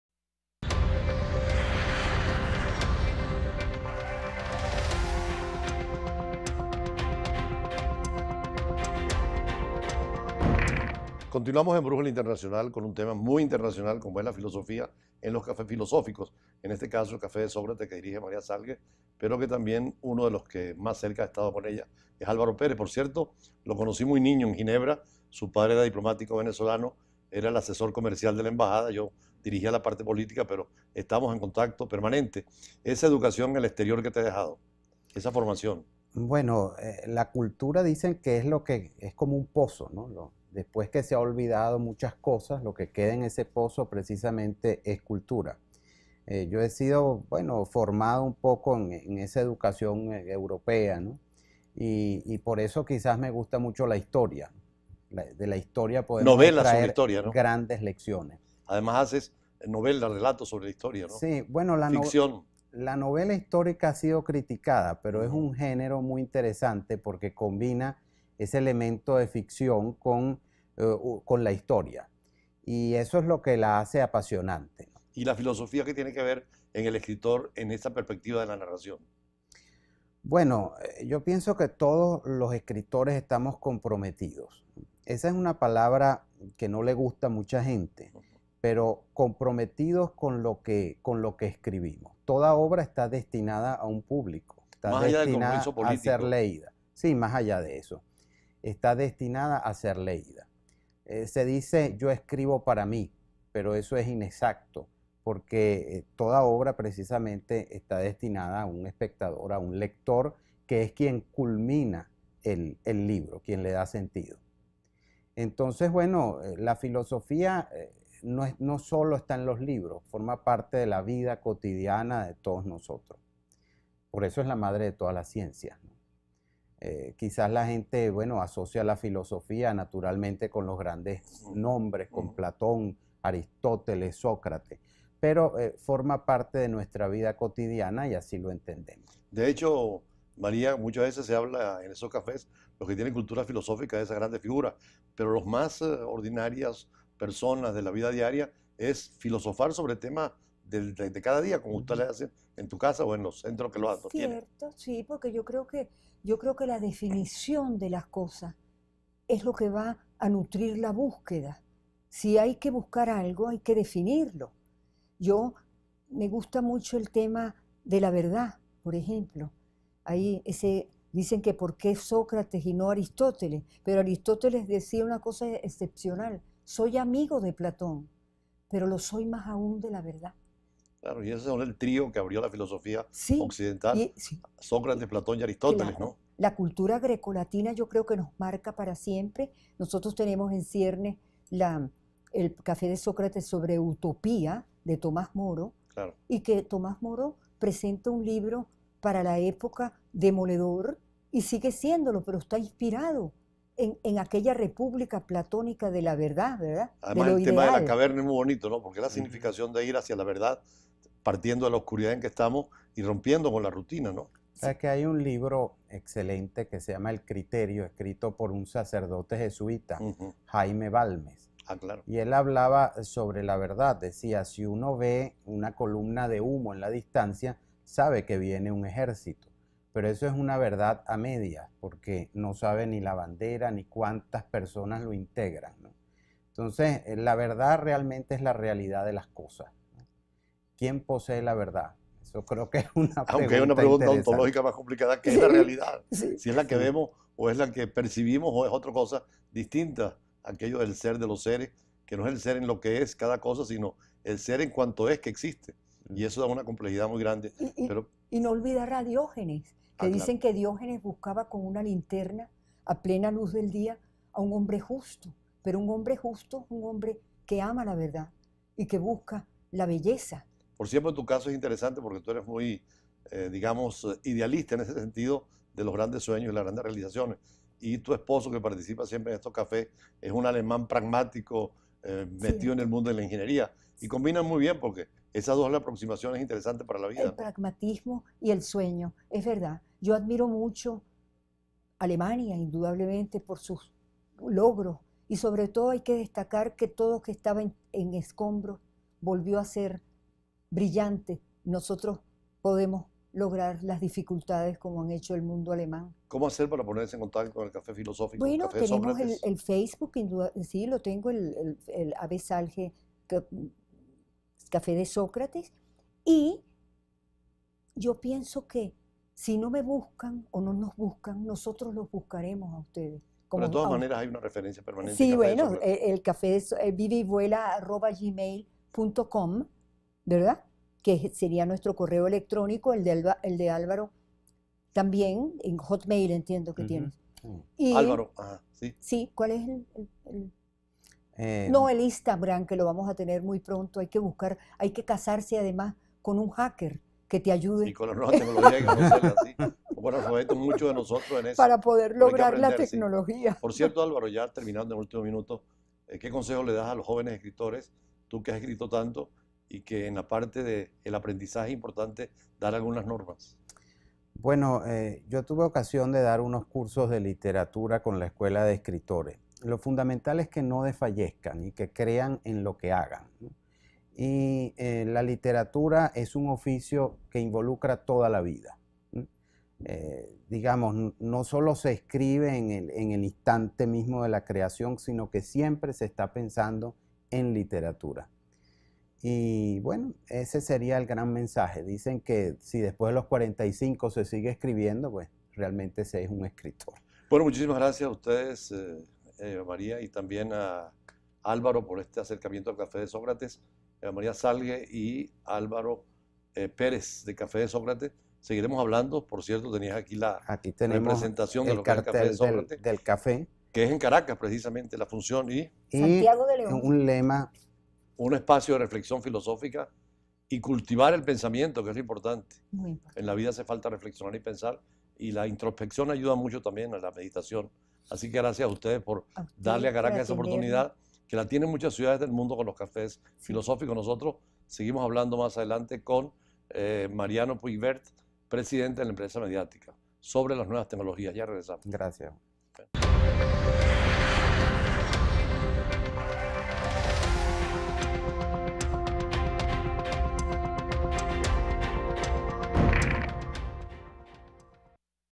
El Café de Sócrates: Filosofía y cultura en Caracas (1/2)
La finalidad de los encuentros va más allá de sostener tertulias sobre temas sociales, filosóficos, científicos o literarios. l Café de Sócrates nació bajo el ideal de rescatar la ética y los principios esenciales de la convivencia fraterna y del respeto más genuino a los derechos humanos